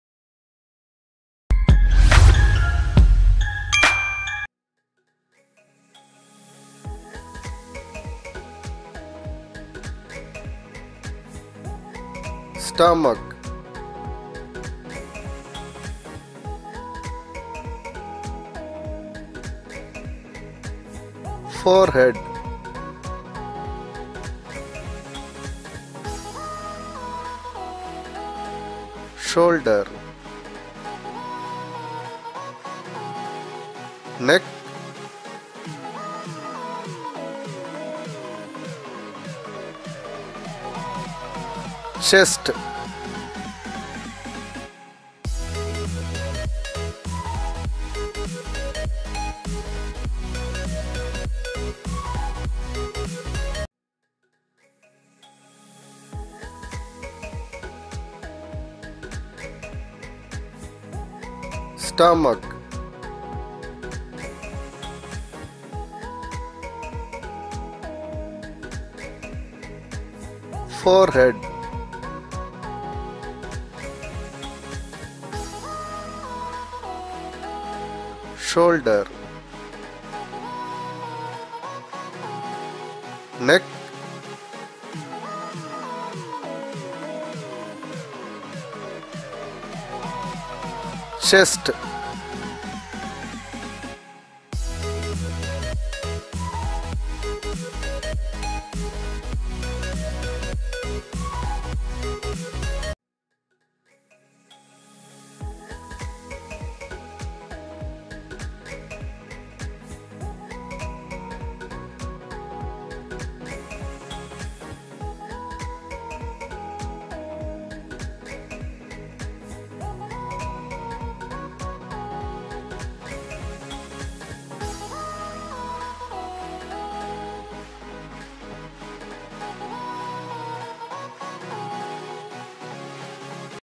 Parts of the body for kids | Parts of body for kids #4 | All Is Well
ALL IS WELL proudly presenting the names of parts of the body, Drawing with exact english pronunciation each video 5 parts, Keep watching... ALL IS WELL every friday one child educational video came.